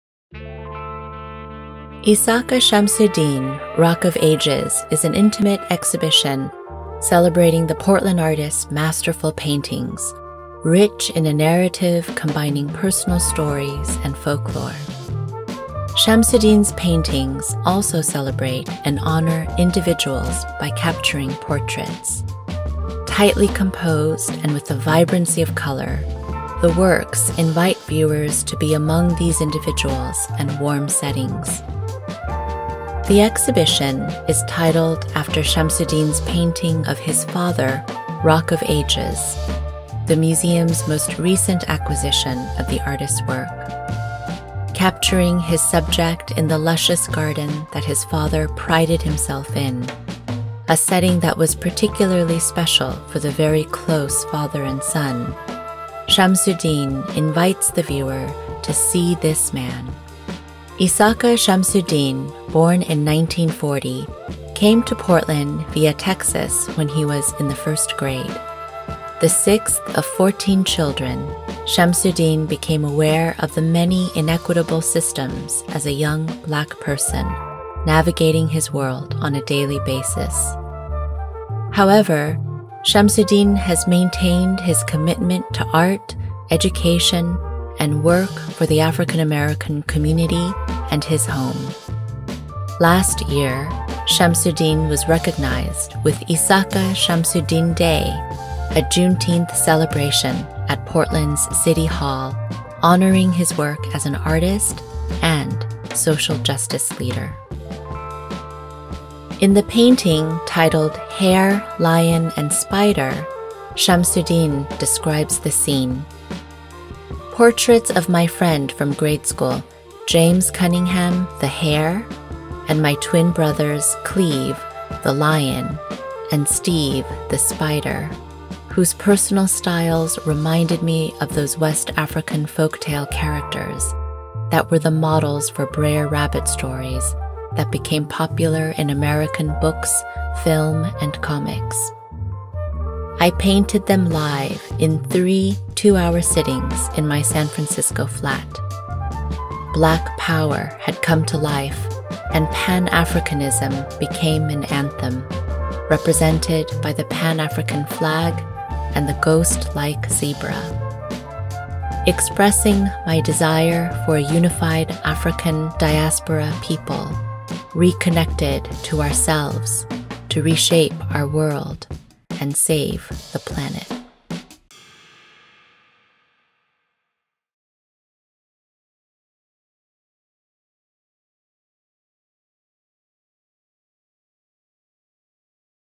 VIRTUAL WALK-THROUGH: Isaka Shamsud-Din: Rock of Ages
Join Grace Kook-Anderson, the Arlene and Harold Schnitzer Curator of Northwest Art, for a deeper look into Rock of Ages, an exhibition highlighting the work of Portland artist Isaka Shamsud-Din. Even though the Museum has reopened, this exhibition is in a gallery that is still temporarily closed. However, you have likely seen Shamsud-Din's work around Portland, as he has painted murals throughout the city.

With special thanks to the partnership of Teressa Raiford of Don't Shoot Portland and the Regional Arts and Culture Council (RACC)

Exhibition Photography by Ben Cort